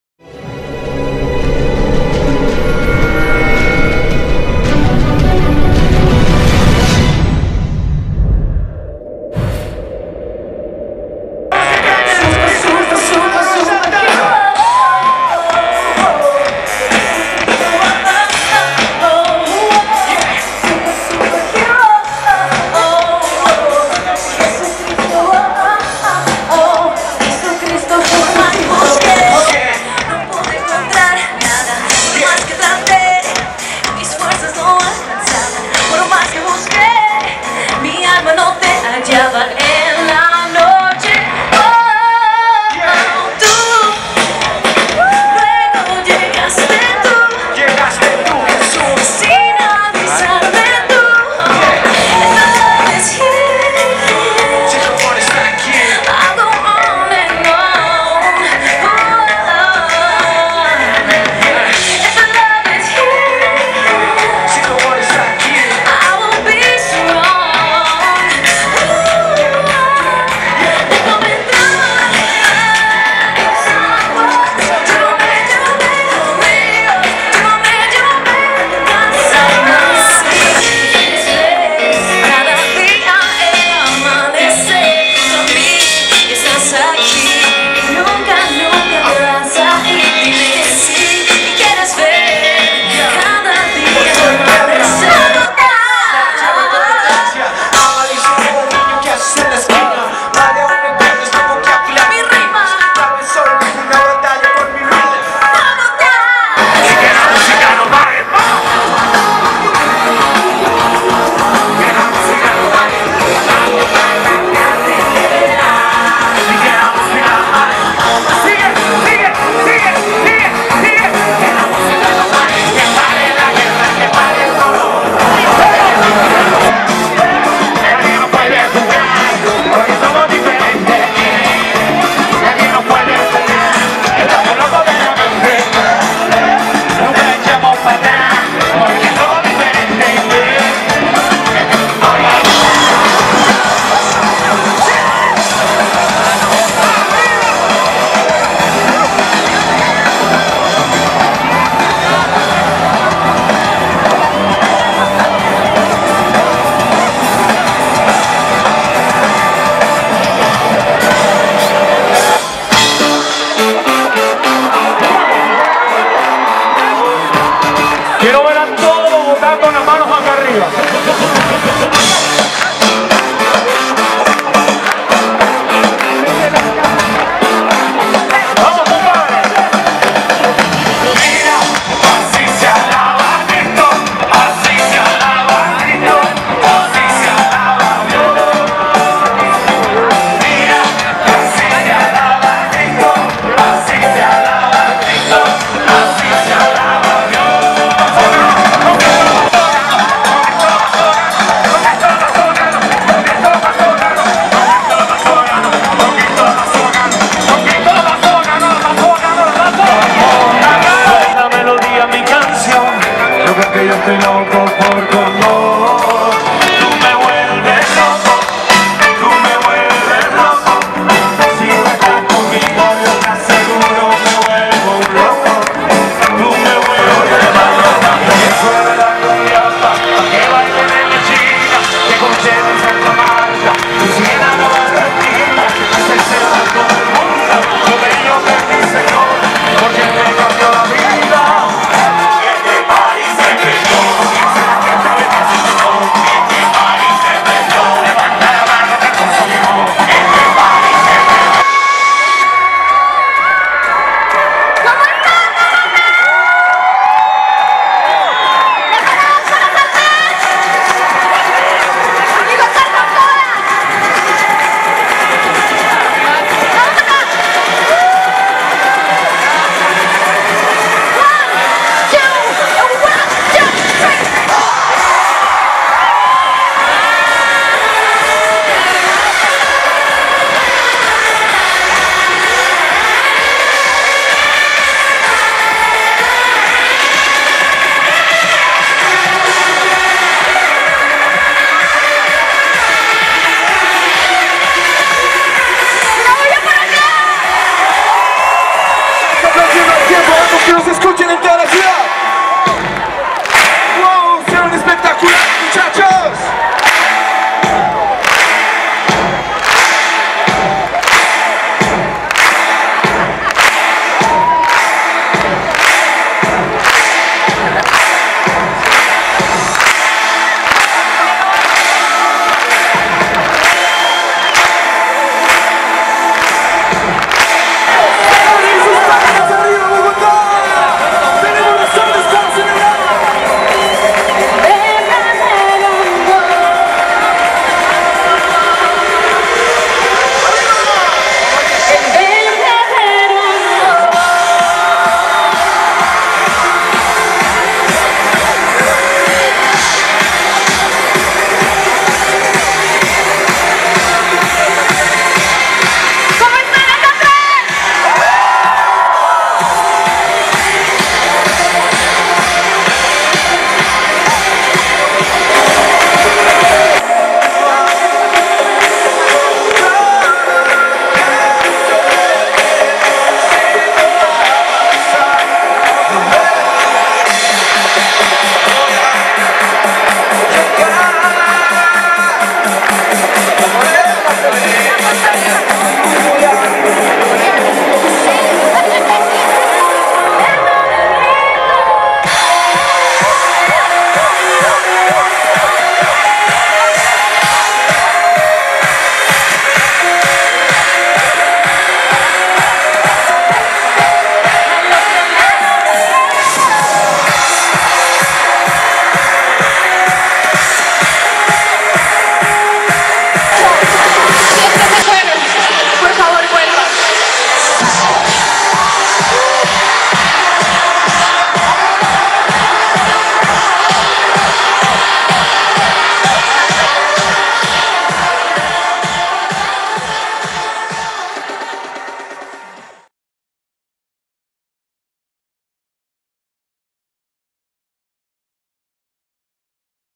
BOGOTÁ GOSPEL 2018
Grabado el 07 de Agosto, durante el 9° Bogotá Gospel 2018, en el Parque Metropolitano Simón Bolívar.